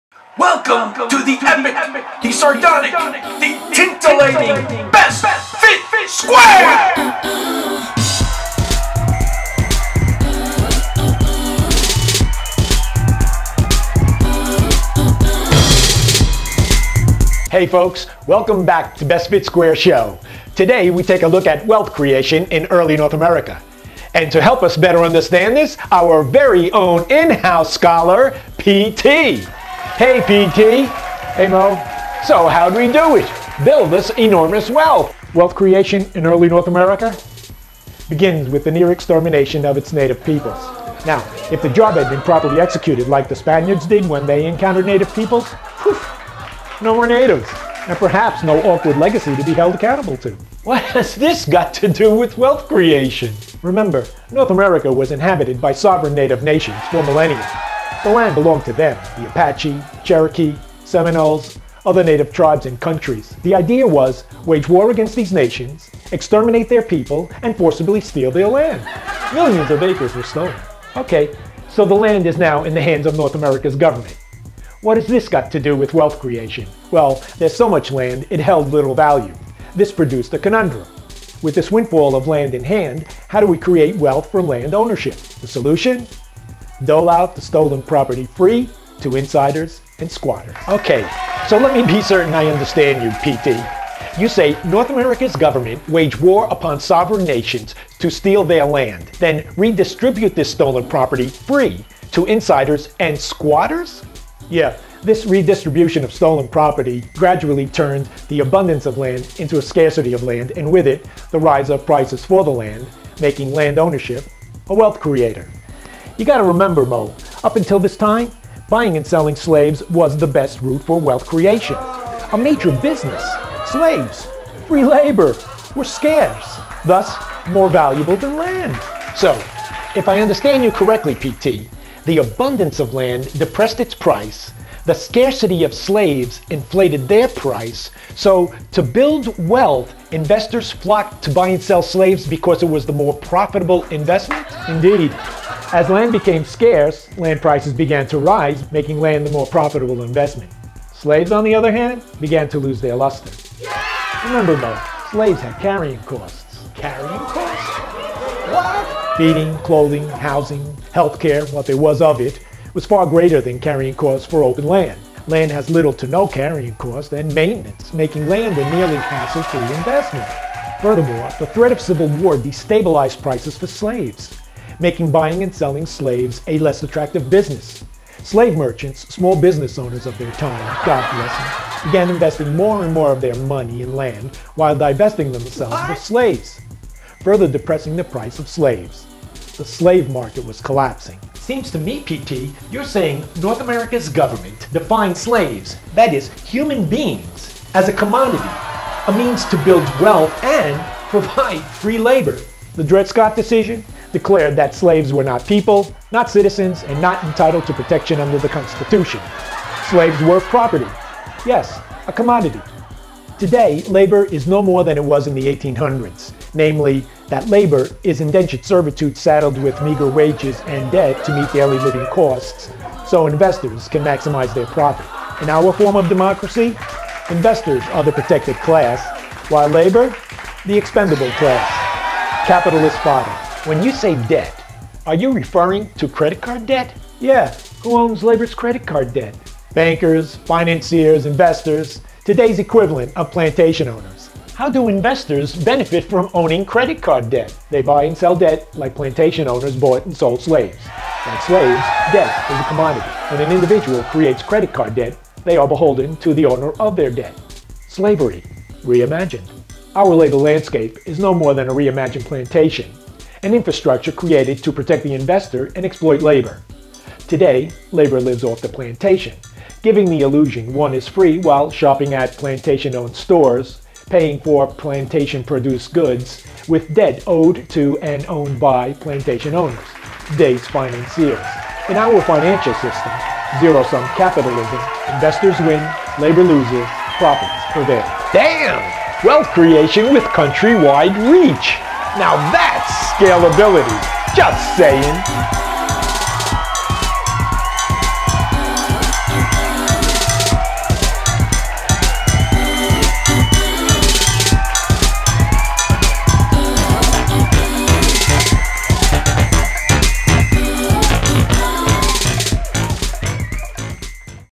Money, S1E3
Join MO and PT in this gigglelicious session as they discuss wealth creation in early north america. you'll laugh your ass off when you learn the role BIG government played doling out favors to insiders like clowns flopping about a three ring circus. Join the cognoscenti...